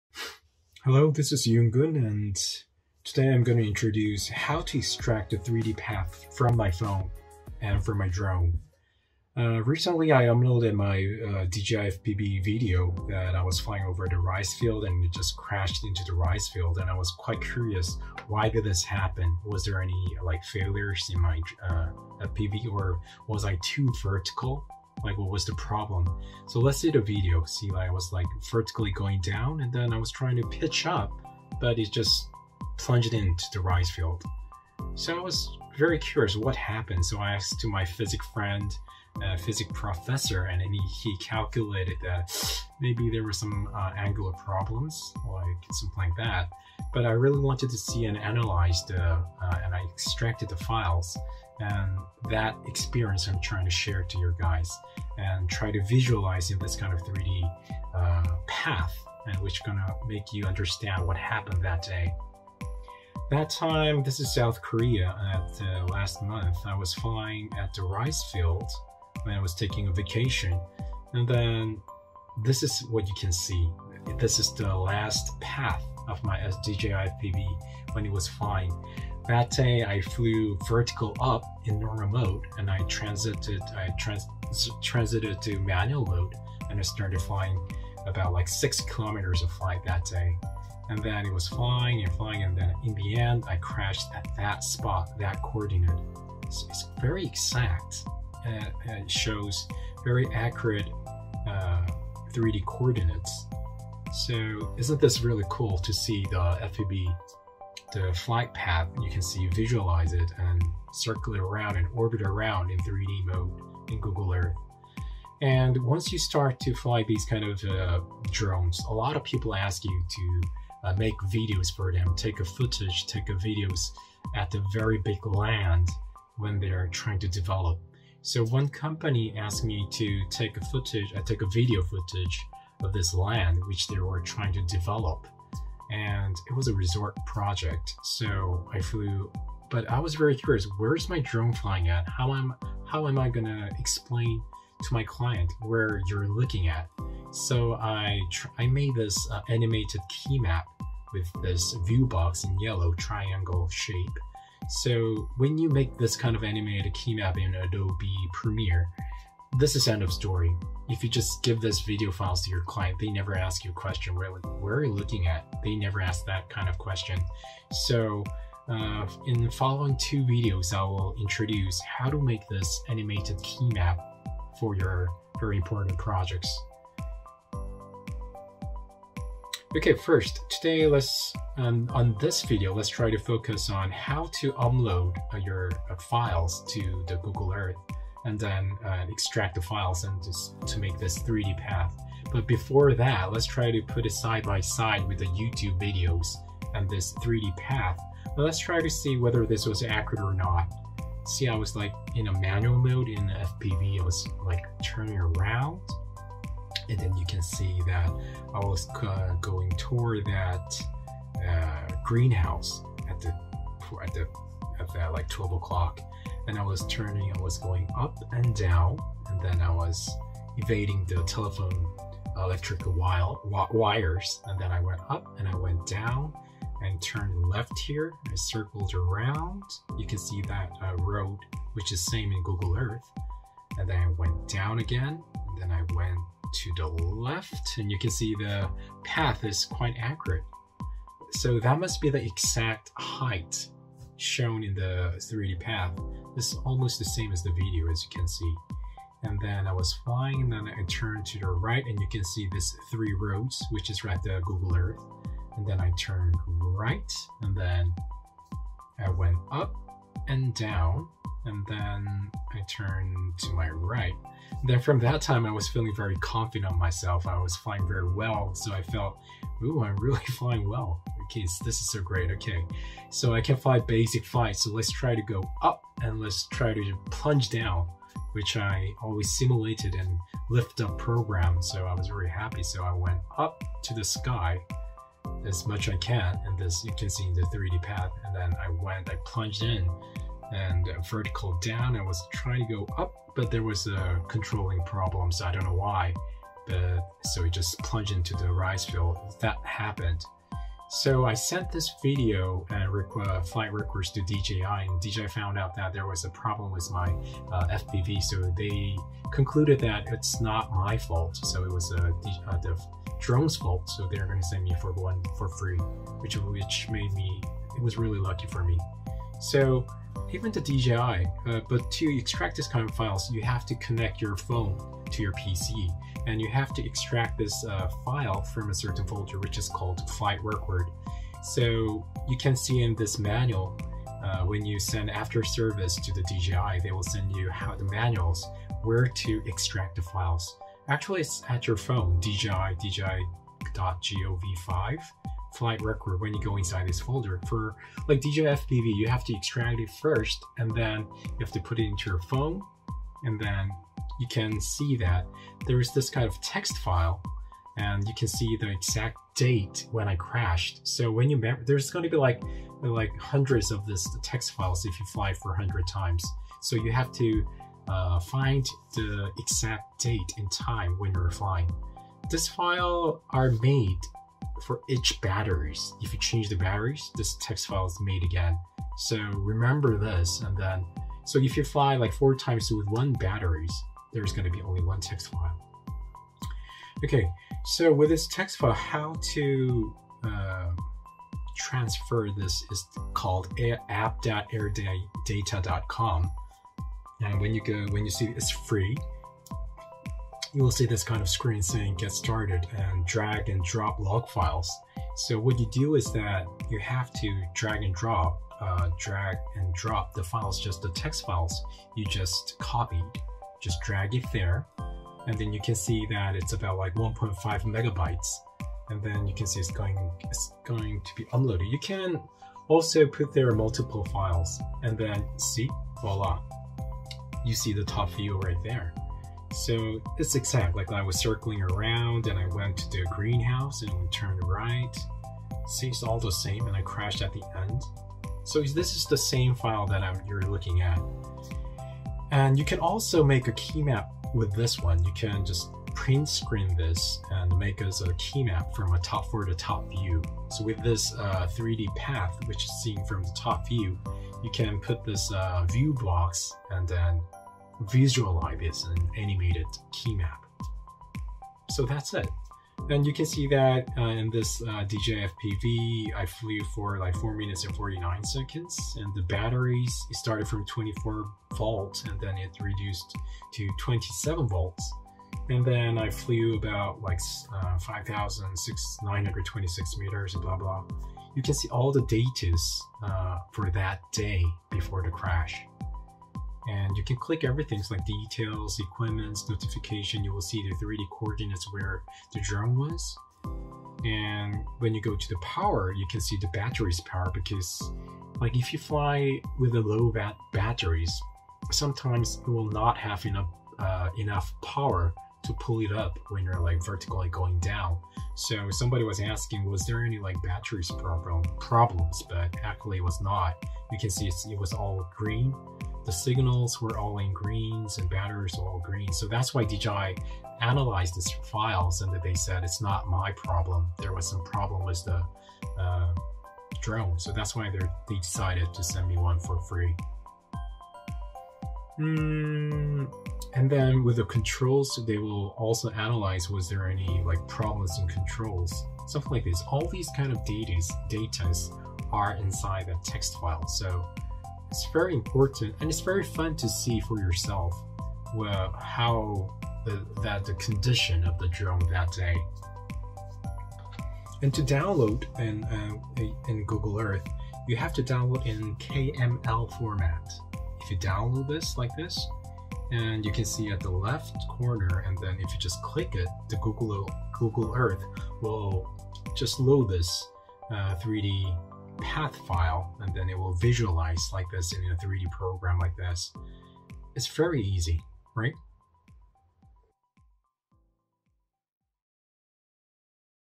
[DJI FPV]-How to Make the Drone's 3D Flight Path in Google Earth
How to make the 3D Flight Path of your drones(dji) by using

1. Google Earth PC
3. dji flight record

procedures
1. download the flight record.txt from your phone
3. export in kml format
4. open the kml file in google earth
5. done